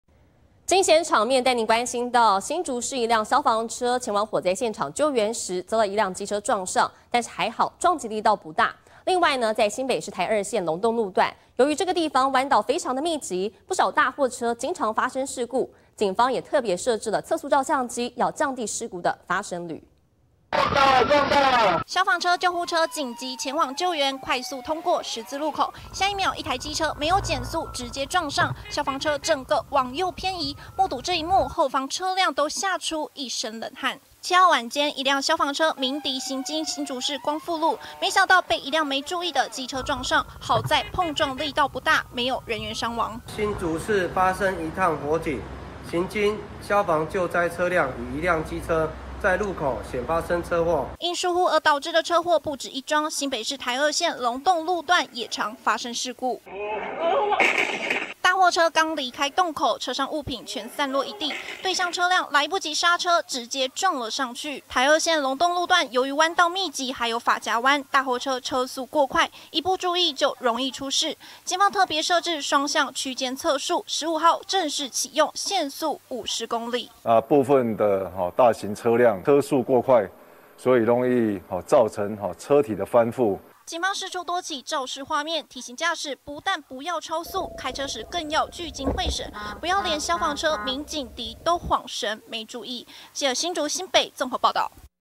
要撞到了！消防車救火途中 遭機車直接撞上
7號晚間，新竹市一輛消防車前往火災現場救援，行經光復路口時，沒想到，被一輛機車沒注意撞了上來，相當驚險，好在撞擊力道不大。另外，新北市台2線龍洞路段，由於彎道密集，不少大貨車經常發生事故，警方也特別設置測速，要提高駕駛注意，降低事故發生。